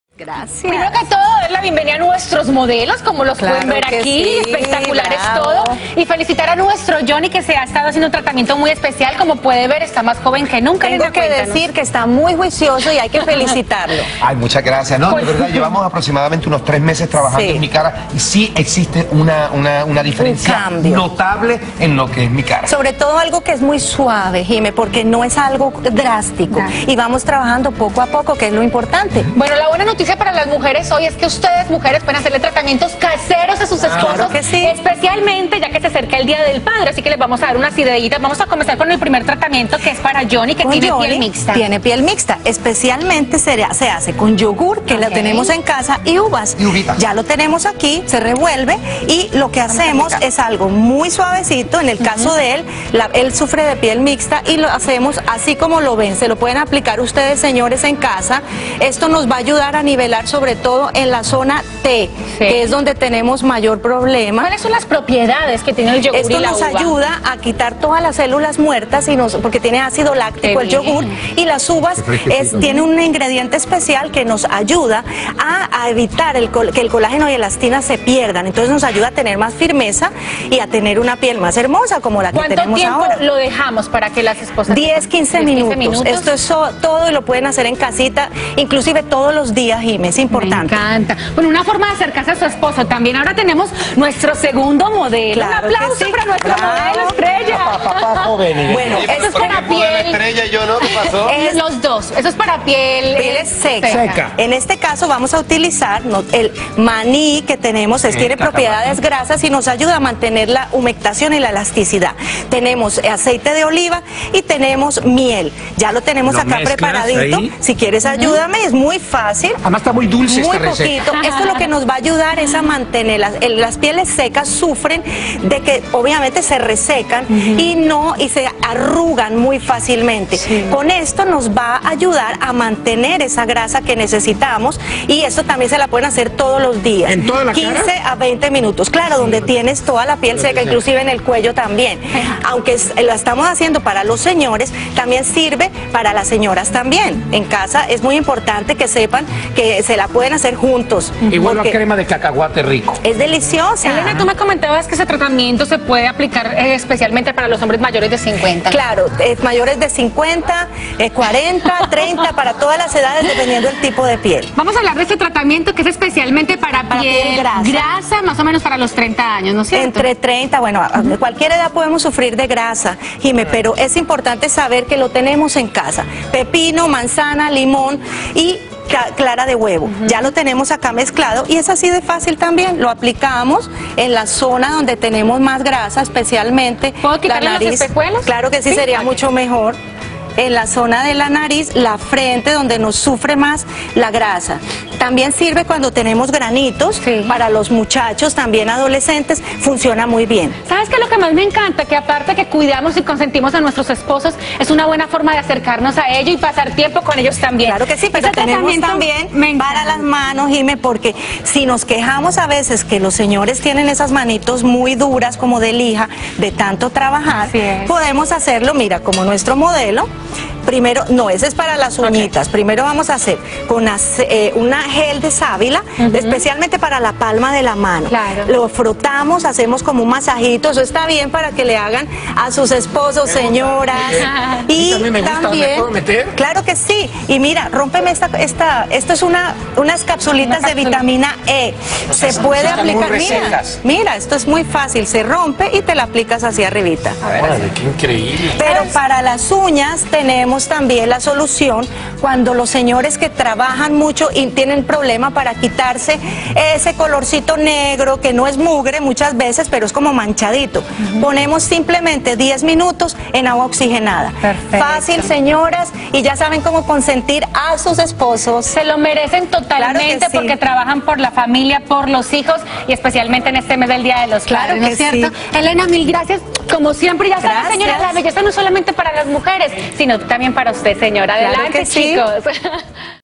¡Los hombres también son vanidosos! Mira estas recetas caseras para cuidar su piel
Nuestros modelos estrella Alan y Johnny demostraron que los hombres también son vanidosos. Poniendo a prueba estas recetas caseras de Helena Sir.